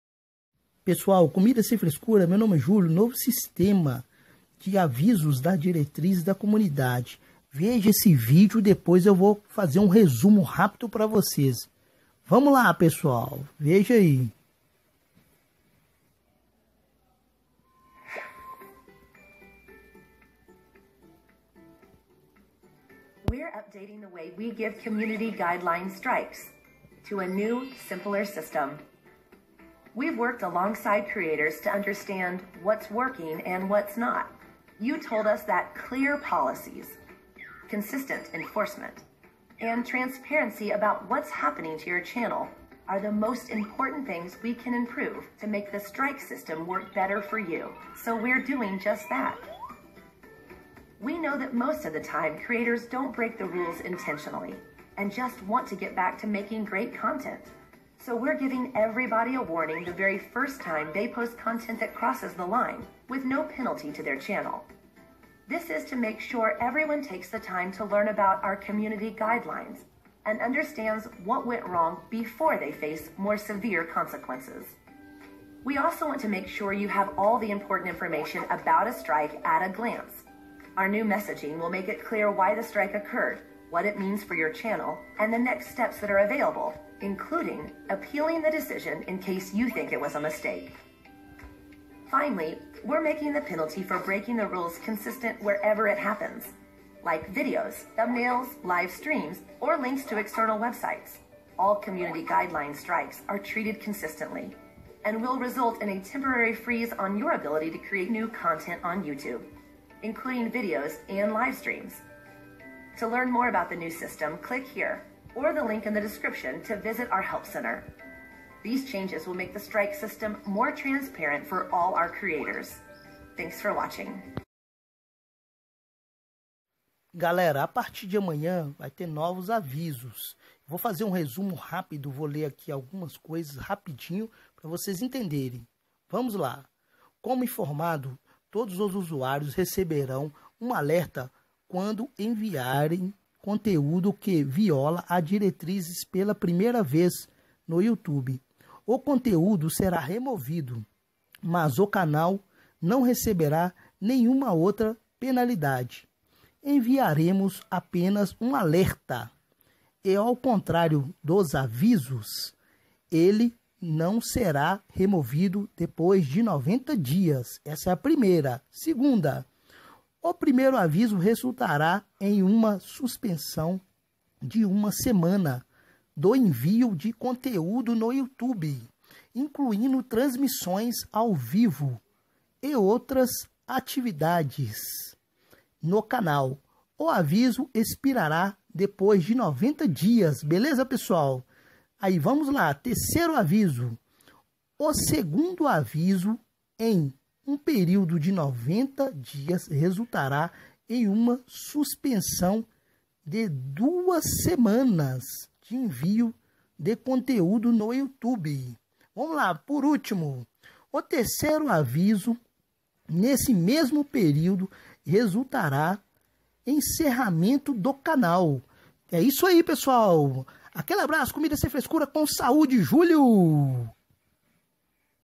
Novo sistema de avisos das diretrizes das comunidades a partir dia 25 fevereiro 2019
Pessoal venho mostra para vocês esse novo sistema do youtube fiz um resumo do que vai acontecer a partir de amanhã, se você quiser sabe muito mais a respeito entre no seu computador aperte no seu logo tipo na sua foto e aperte em seguida (youtube studio beta ) aí vai aparecer novidades do sistema de avisos do youtube ok espero que tenha entendido caso tenha mais dúvidas procurar também canais que tenha vídeos de tutoriais ,estou sempre aqui para ajudá meus inscritos com novidades espero que gostem.
Obrigado Comida sem frescura com saúde abraços do JULIO bacana de mais.